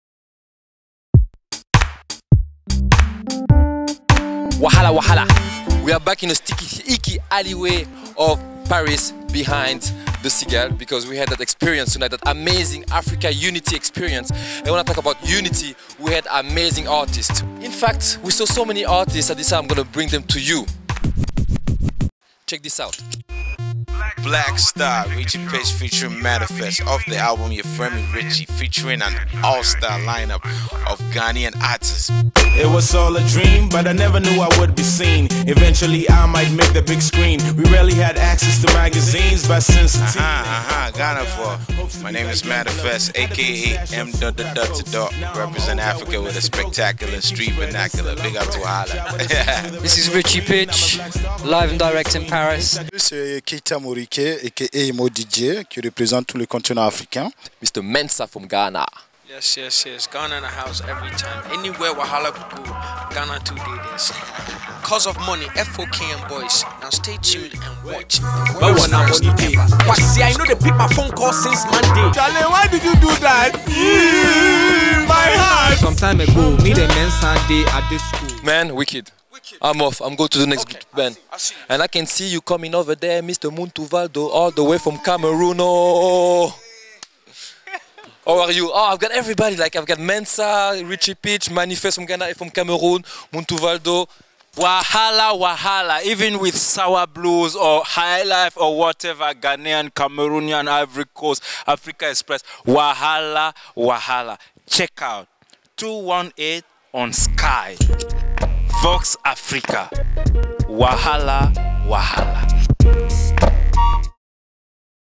Teaser WAHALA feat m.Annifest, Ritchy Pitch, MO DJ, Mensah, Muntu Valdo
Finger on the pulse, WAHALA keeps documenting the AFRO cultural revolution in rea-time, we are in the sticky-icky alley way of urban Paris where ERIC SOUL catch the AFRO elite relaxing and chilling after their Puma's AFRICAN EXPRESS show at the CIGALE!